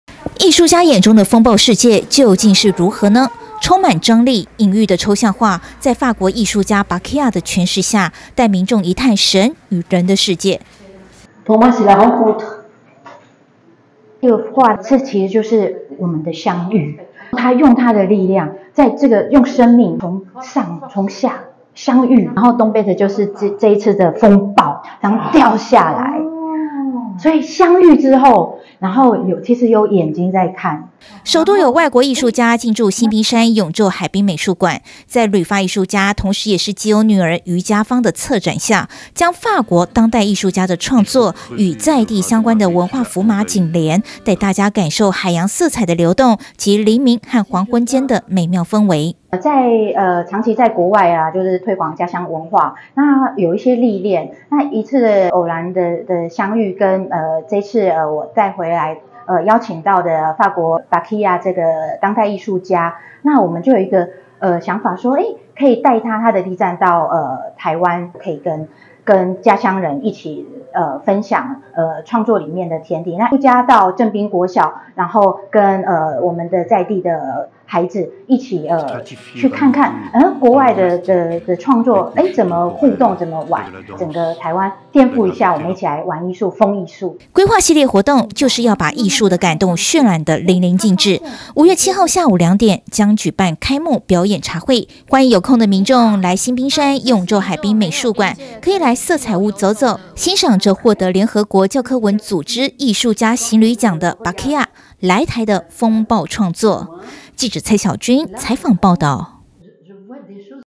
當代 藝術|法國藝術家駐村策展 開啟抽象藝術風暴世界
(中嘉吉隆新聞 記者蔡曉君/基隆報導) 基隆星濱山永晝海濱美術館，5/7~5/28進駐第一位外國藝術家 Akila Benyahia，擅長以多元複合式媒材創作的Bakila來自法國，得獎無數，在旅法藝術家也是基隆女兒余佳芳的策展下，帶大家進入以風暴為主題的抽象畫世界，歡迎有空的民眾前往欣賞。

藝術家眼中的風暴世界究竟是如何？充滿張力、隱喻的抽象畫，在法國藝術家Bakila的詮釋下，帶民眾一探神與人的世界。

首度有外國藝術家進駐星濱山永晝海濱美術館，基隆女兒余佳芳旅居法國多年，推展台灣基隆的中元祭等文化傳統與藝術活動，這次回國邀請Akila Benyahia帶來抽象當代藝術，將法國當代藝術家的創作與在地相關的文化符碼緊連，要帶大家感受海洋色彩的流動及黎明和黃昏間的美妙氛圍。

還要帶藝術家到正濱國小與師生交流，佳芳規劃系列活動，就是要把藝術的感動渲染得淋漓盡致，5/7下午兩點將舉辦開幕表演茶會，歡迎有空地民眾來星濱山永晝海濱美術館，可以來色彩屋走走，欣賞這獲聯合國教科文組織“藝術家行旅獎“的Bakila ，來台的風暴創作。

臉書追起來
Home 中嘉- 吉隆有線電視

趣聞不漏接
吉隆官網(中嘉新聞)

喜歡看電視?